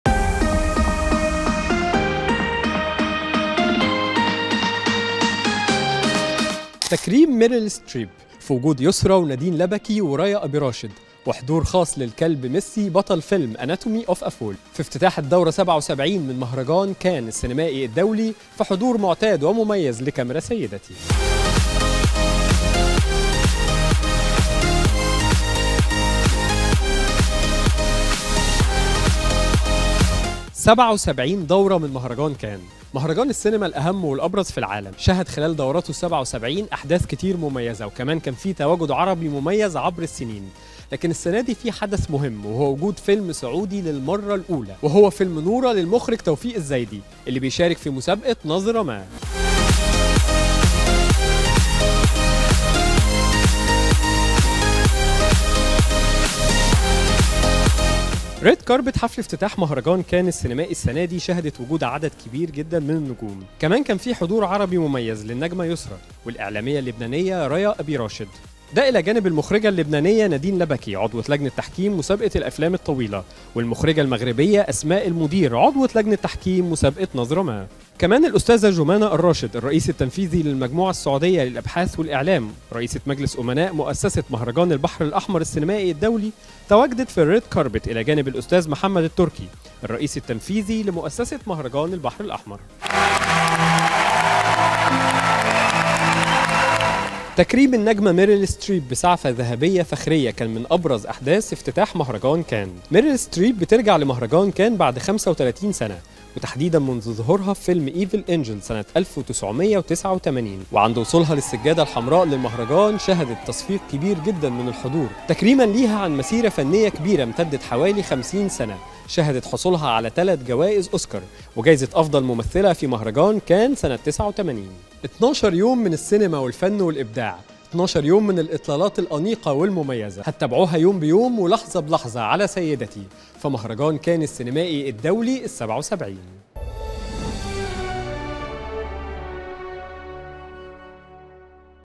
افتتاح مهرجان كان السينمائي في دورته الـ77، بحضور كاميرا سيدتي.. المزيد في نشرة اليوم الأول
افتتاح
في دورته الـ77، بحضور معتاد ومميز لكاميرا سيدتي.. شهد حفل الافتتاح تكريم ميريل ستريب في وجود يسرا ونادين لبكي وريا أبي راشد وحضور خاص للكلب ميسي بطل فيلم Anatomy of a Fall.. المهرجان في هذه الدورة يشهد حدث مميز وهو تواجد الفيلم السعودي
المزيد من التفاصيل في نشرة اليوم الأول